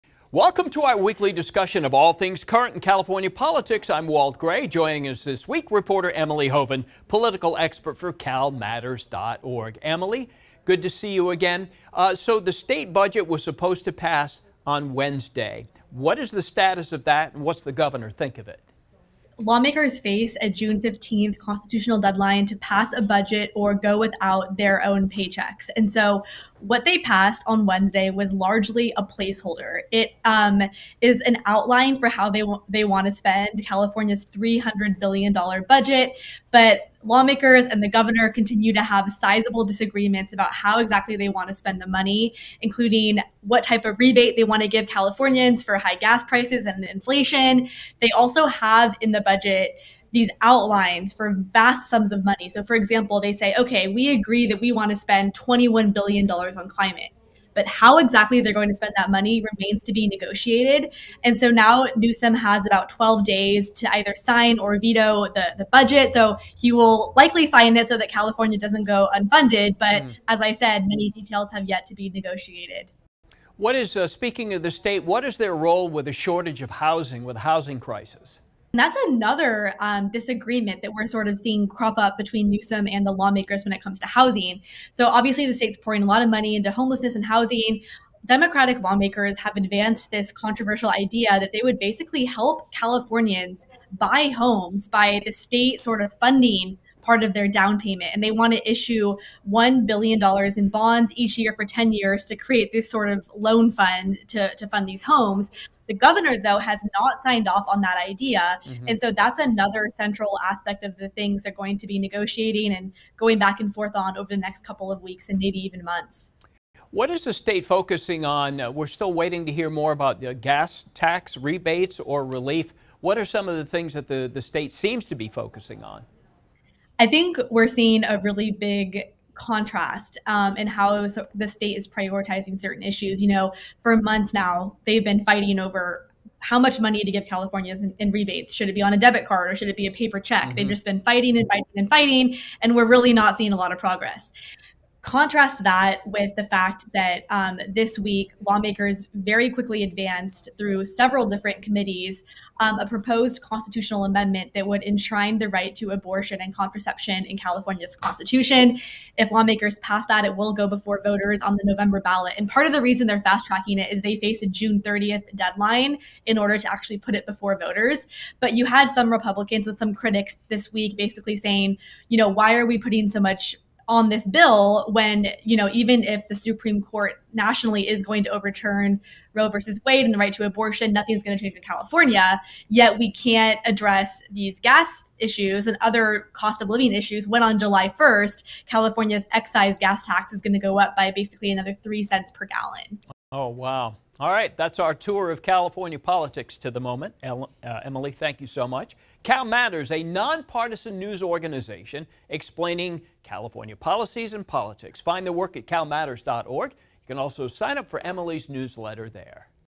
California Politics: Latest on the state's budget, possible relief for home buyers
CalMatters political reporter Emily Hoeven and ABC10’s Walt Gray discuss the biggest headlines in California politics this week.